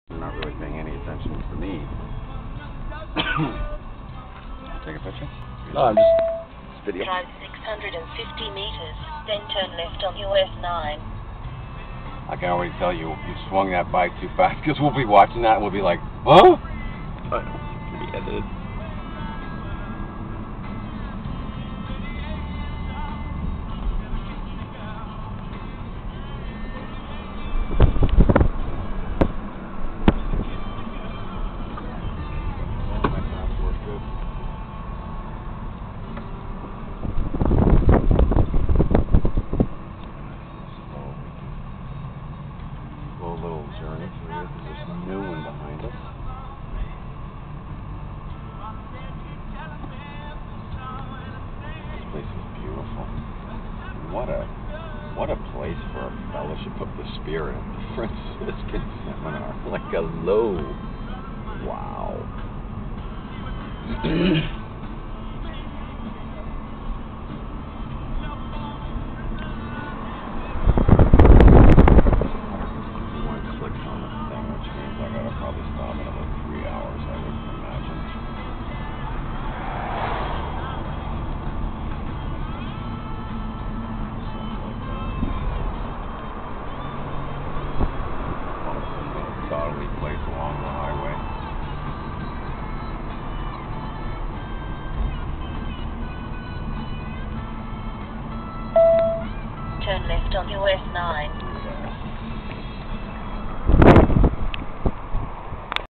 fransiscan Monastary in The Catskill mountains
this is the grounds of the Franciscan monastery i visited one weekend for a retreat. It is in the Catskill mountain in New York State.